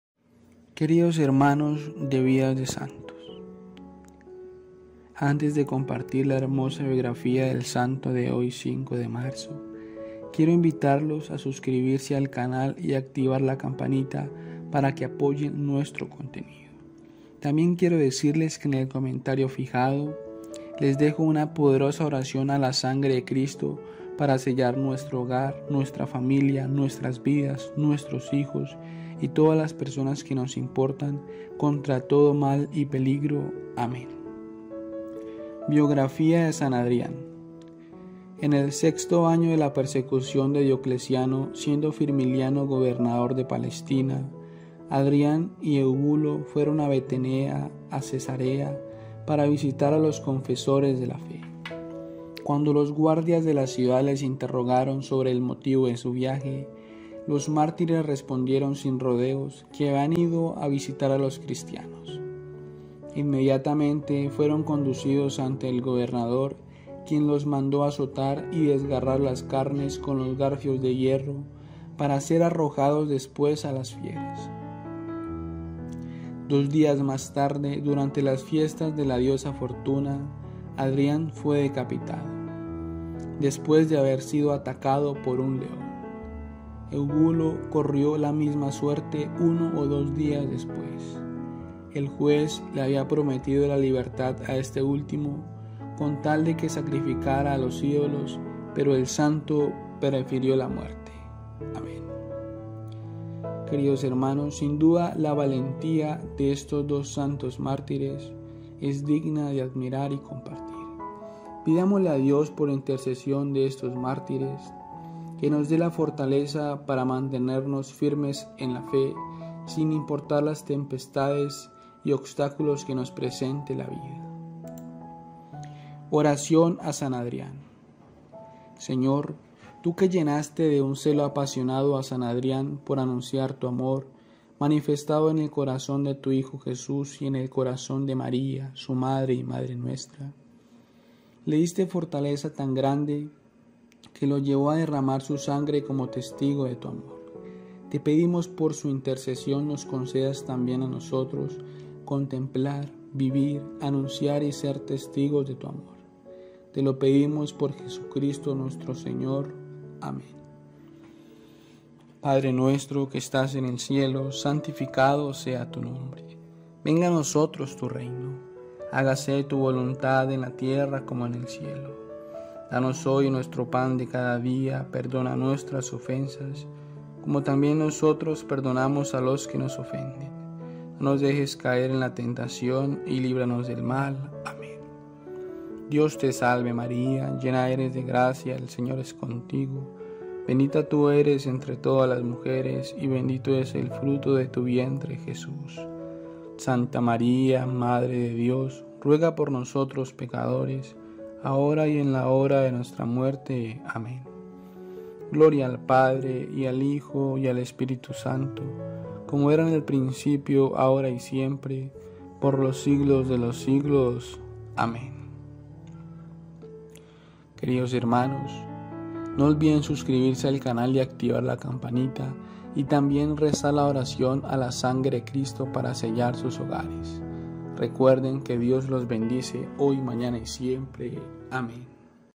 San Adrián Mártir de la Fe Historia Completa 🙏
Historia y biografía de San Adrián.

En el sexto año de la persecución de Diocleciano, siendo Firmiliano gobernador de Palestina, Adrián y Eubulo fueron de Batenea a Cesarea para visitar a los confesores de la fe. Cuando los guardias de la ciudad les interrogaron sobre el motivo de su viaje, los mártires respondieron sin rodeos que habían ido a visitar a los cristianos. Inmediatamente fueron conducidos ante el gobernador, quien los mandó azotar y desgarrar las carnes con los garfios de hierro, para ser arrojados después a las fieras.

Dos días más tarde, durante las fiestas de la diosa Fortuna, Adrián fue decapitado, después de haber sido atacado por un león. Eubolo corrió la misma suerte, uno o dos días después. El juez le había prometido la libertad a este último, con tal de que sacrificara a los ídolos, pero el santo prefirió la muerte.

😲 Oh my God ¿Aún no me sigues? 😲 HAZLO AHORA 👇🏽